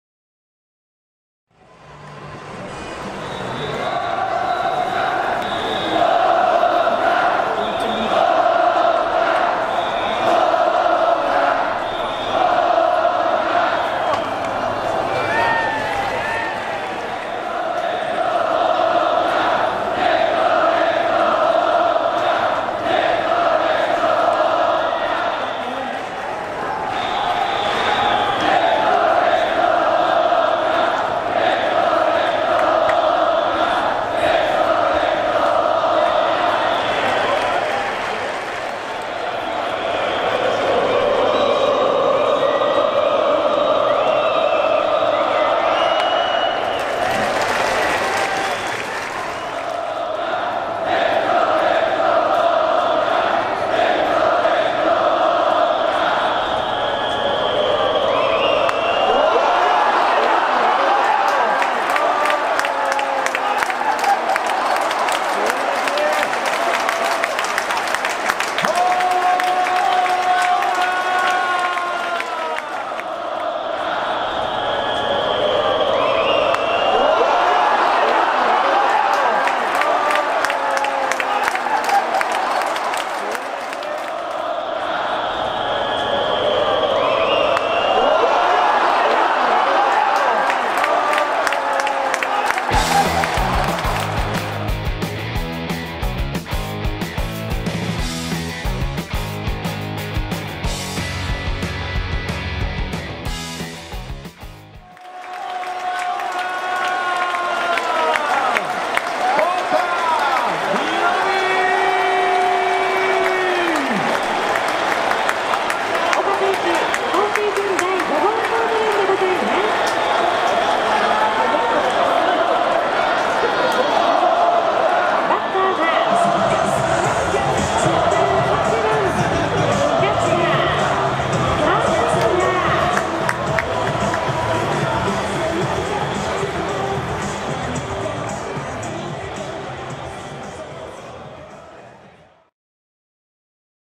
190822 岡大海 アジャもびっくり！？【看板直撃】の超特大アーチ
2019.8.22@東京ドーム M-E
全球ダイジェスト ＊ホームランの場面は【0:55】頃からです。
撮影機材
Panasonic GH4
Panasonic45-175mm
RODE Video Micro
編集
ムービーメーカー
使用音源